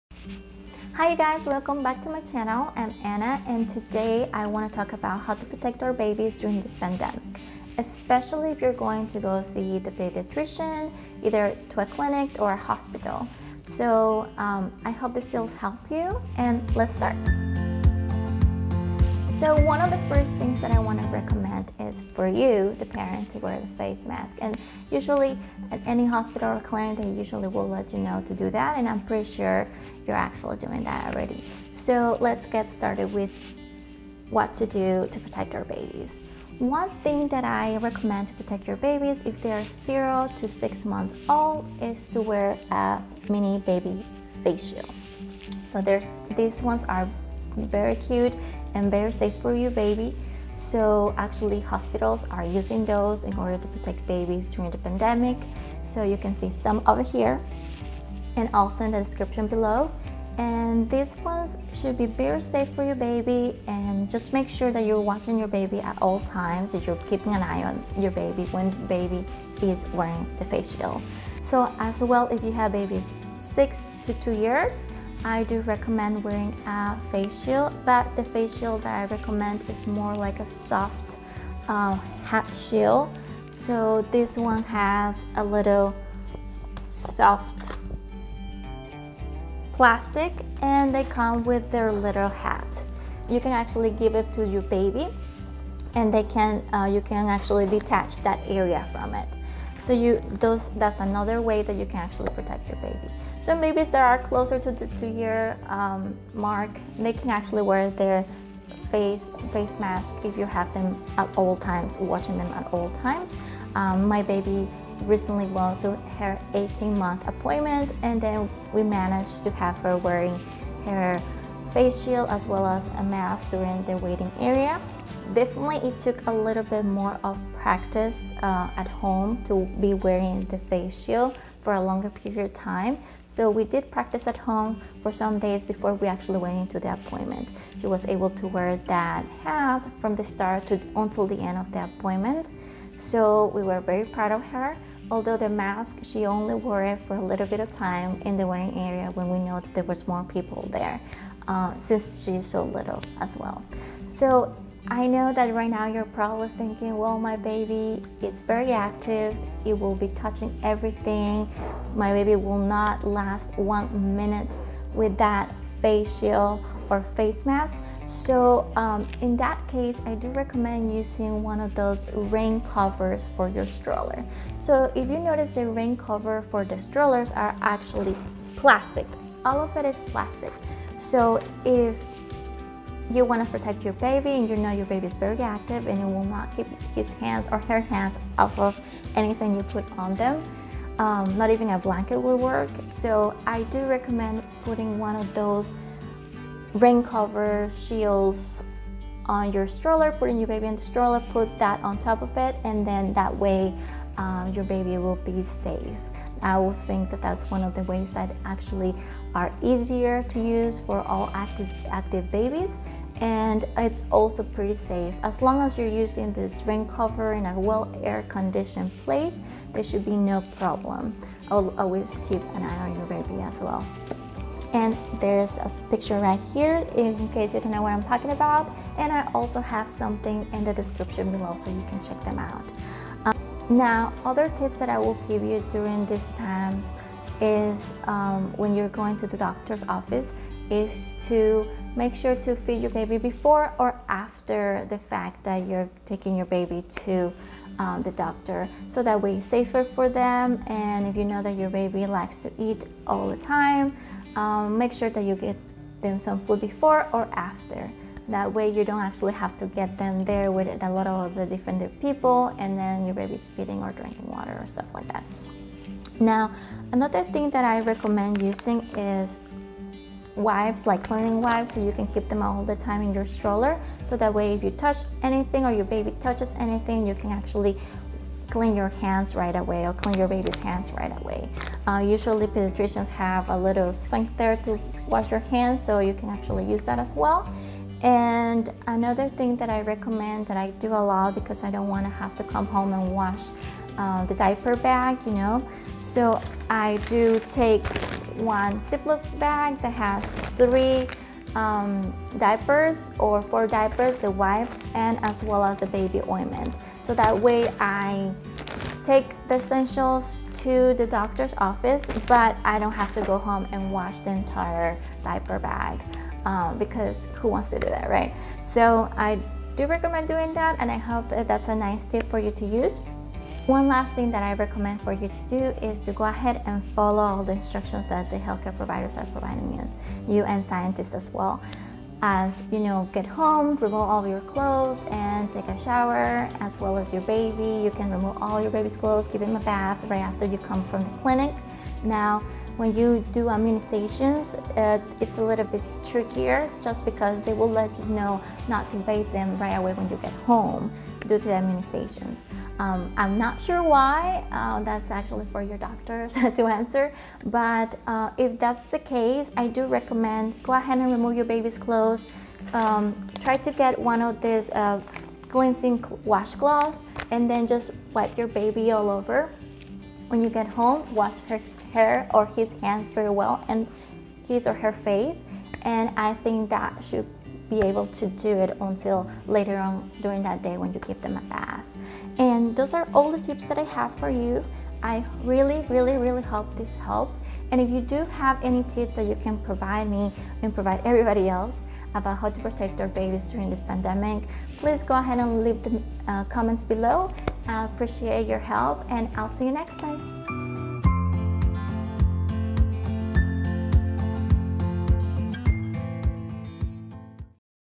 Well Care visits during a Pandemic | Kids Safety Tips
Hi everyone, I wanted to make this video to provide some tips on how to protect our babies. No matter how small they are there are always ways to protect them against this pandemic. These tips apply especially for visits to clinics or hospitals but are also useful in case you need to take your baby to the grocery store with you. Definitely reducing exposure, and limiting large gatherings are key to stay safe during this pandemic so stay safe!

Remember only to use stroller rain covers in a place that is well aired, avoid using in a high-temperature setting like outside if it is hot.

Leaving here some helpful resources!
Mask Safety 101: Nation Wide Children's